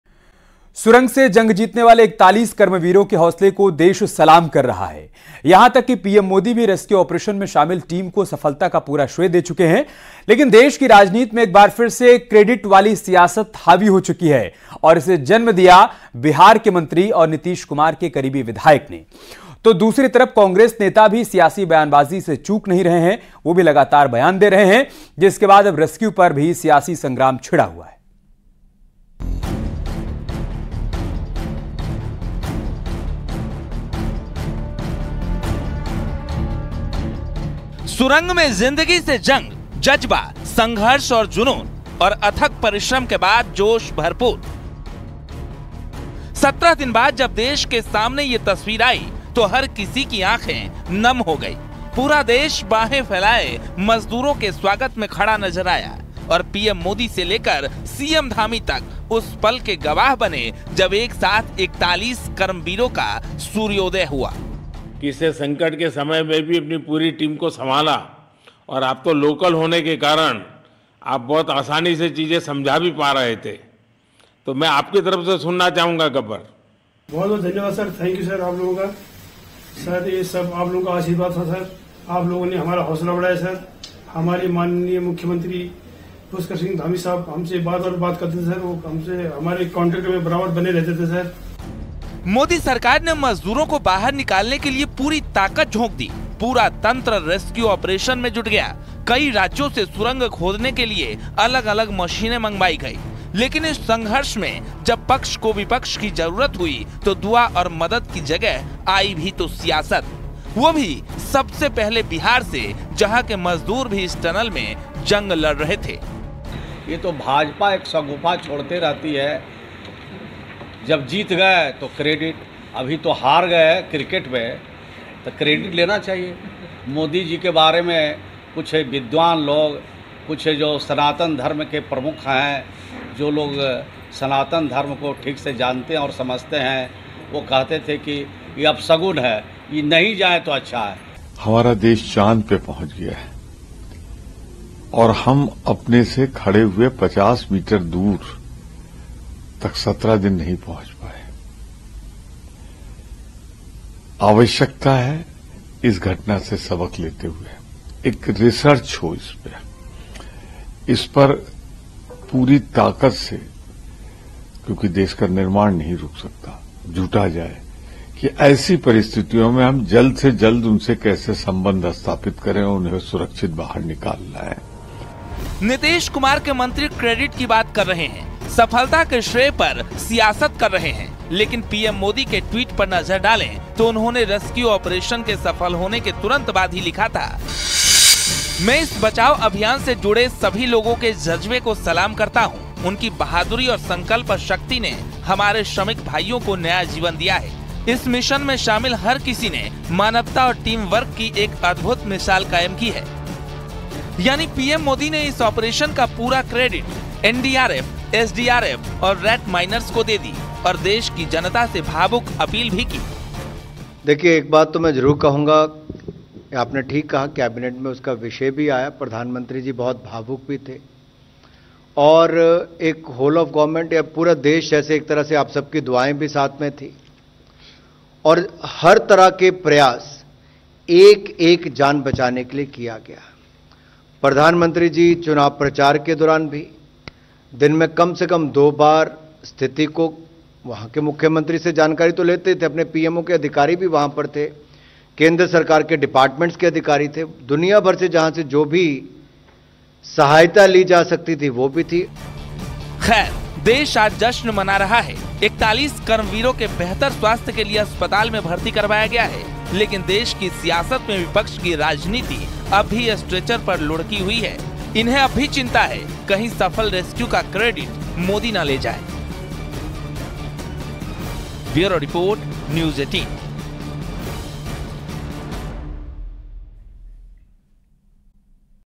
Uttarkashi Tunnel Rescue Updates: मजदूर की नहीं विपक्ष को श्रेय की चिंता?।Congress। JDU। PM Modi
PM Modi ने उत्तराखंड के सिलक्यारा सुरंग से सकुशल बाहर निकाले गए श्रमिकों से फोन पर बातचीत कर उनके स्वास्थ्य और कुशलक्षेम की जानकारी ली है.  दूसरी तरफ बिहार सरकार के मंत्री और सीएम Nitish Kumar के करीबी Shrawan Kumar ने विवादित बयान दिया है. रेस्क्यू ऑपरेशन को लेकर BJP को घेरा है.वहीं, Congress  सांसद Pramod Tiwari ने भी बड़ा बयान दिया है. देखिए पूरी खबर...